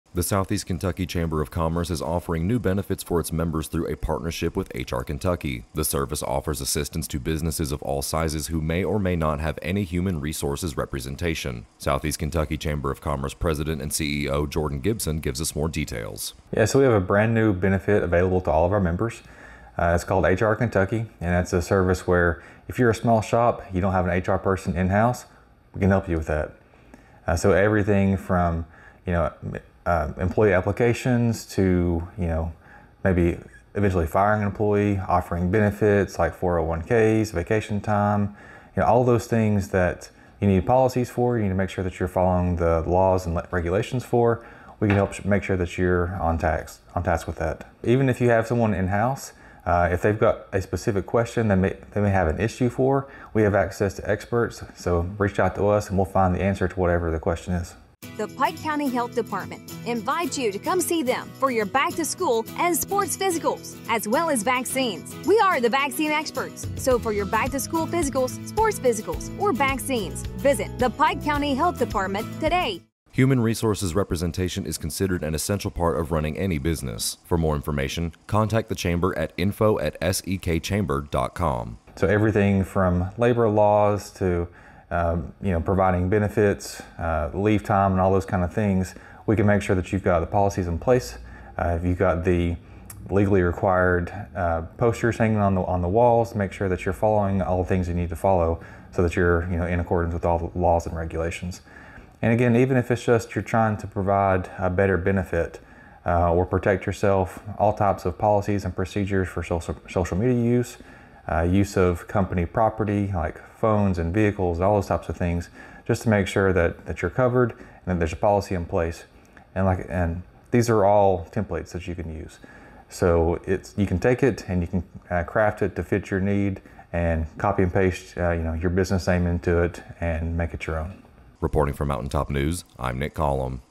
SEKCC is Offering new Benefits Through HR Kentucky
The Southeast Kentucky Chamber of Commerce is offering new benefits to its members through HR Kentucky.